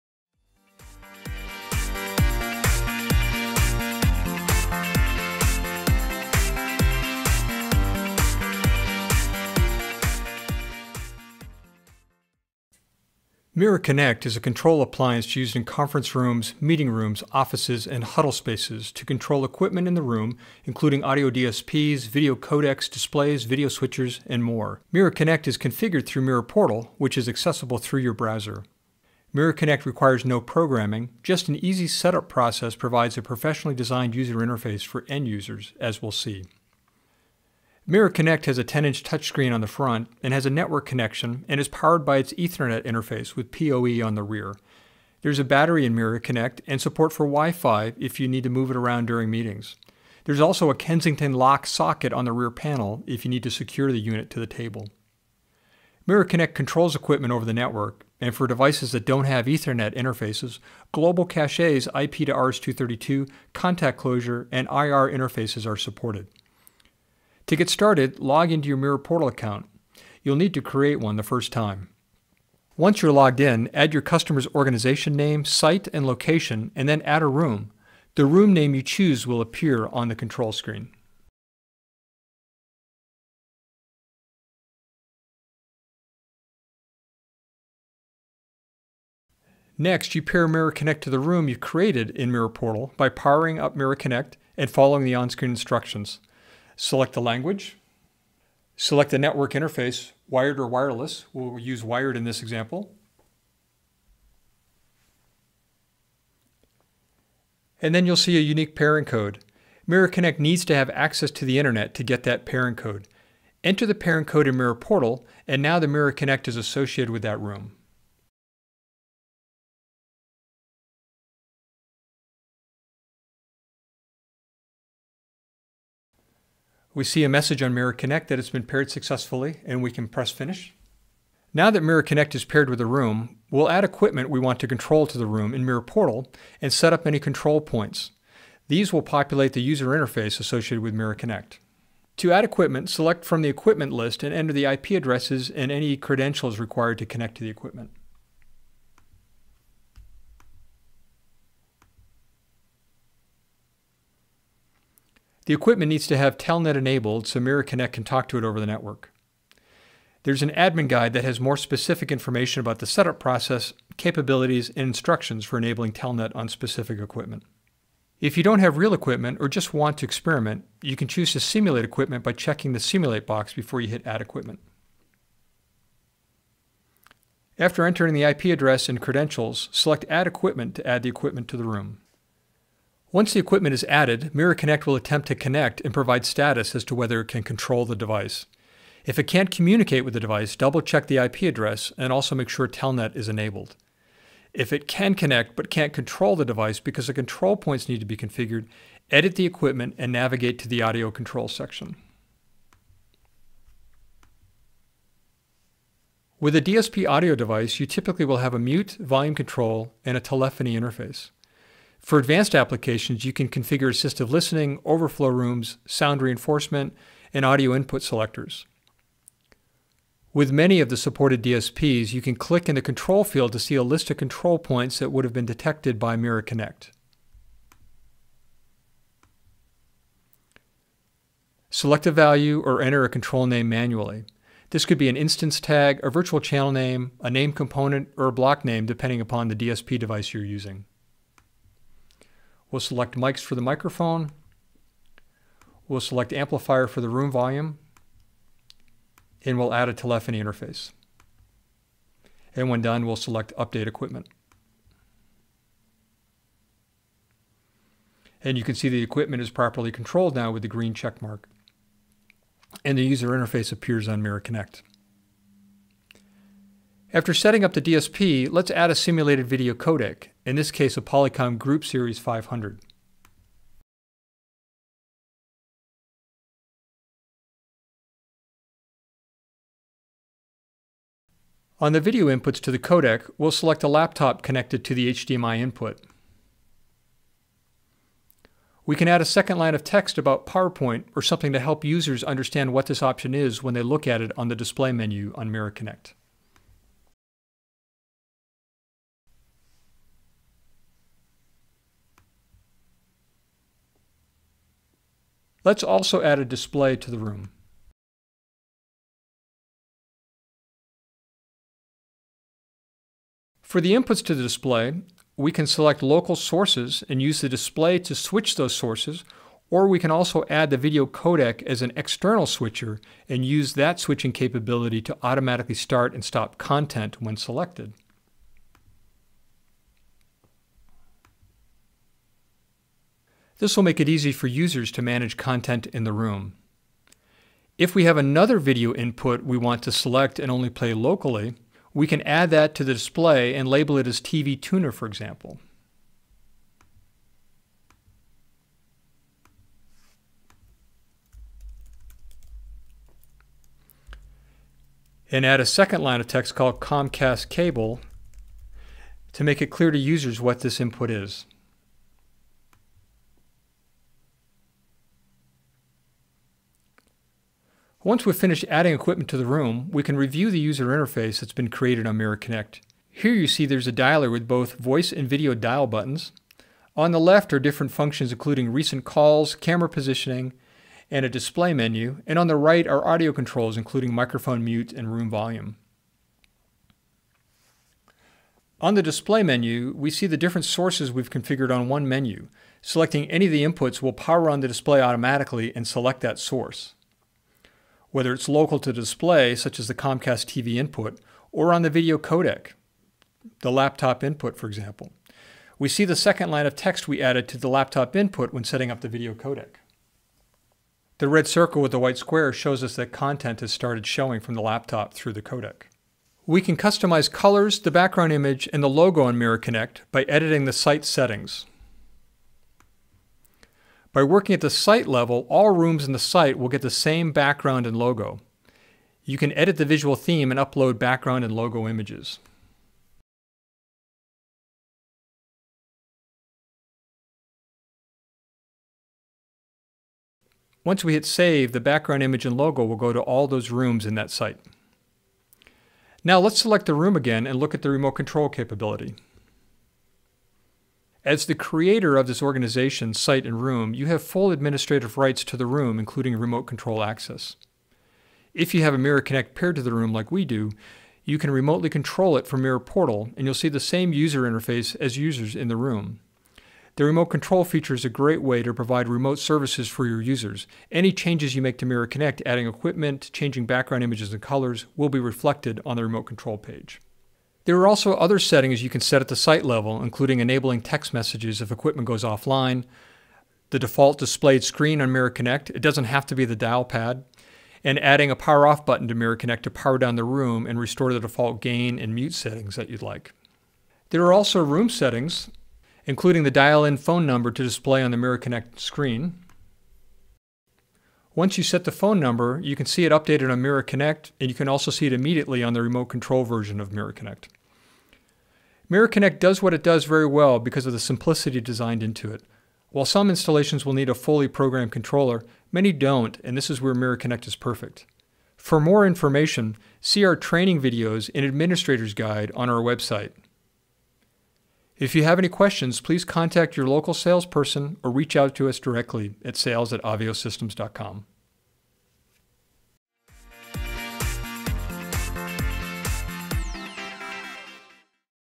Aveo Systems - Mira Connect Demonstration
Full credit for the video goes to Aveo Systems, below is a link to their channel.

Check out this demonstration on how you set up your Mira Connect system with Mira Portal.